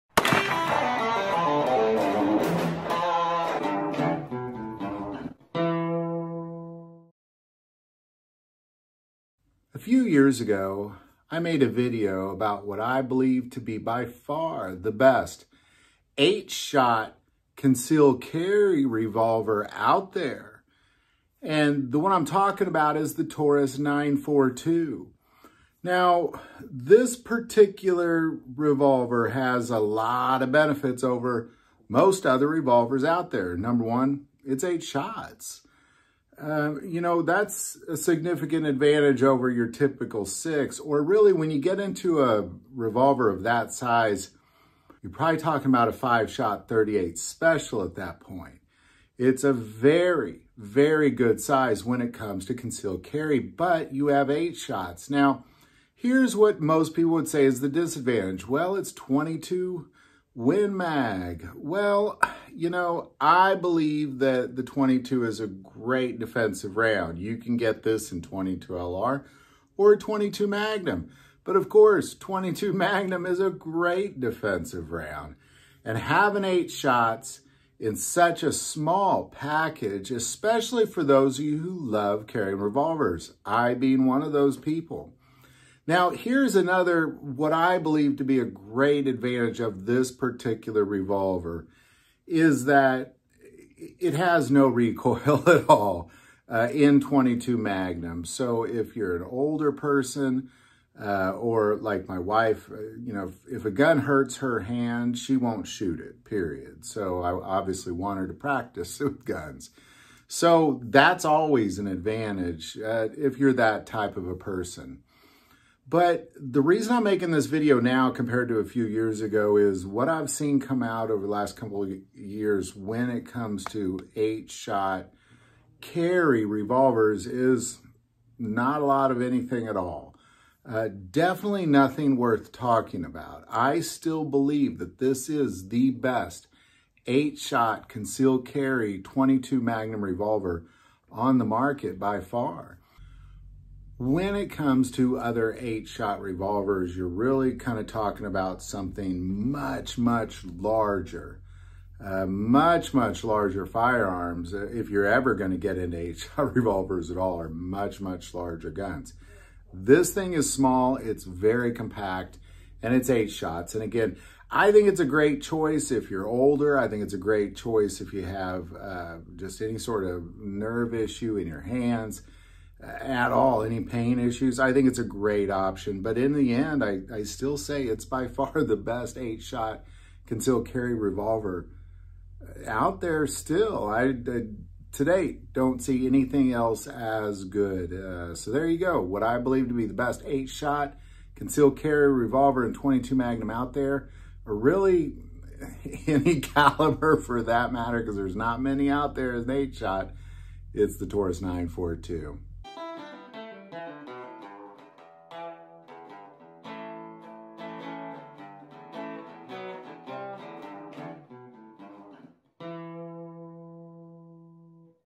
Still The Best 8 Shot CCW Revolver!!
This video takes a look at the BEST 8 Shot Revolver for Concealed Carry!  The Taurus 942 in 22 Magnum!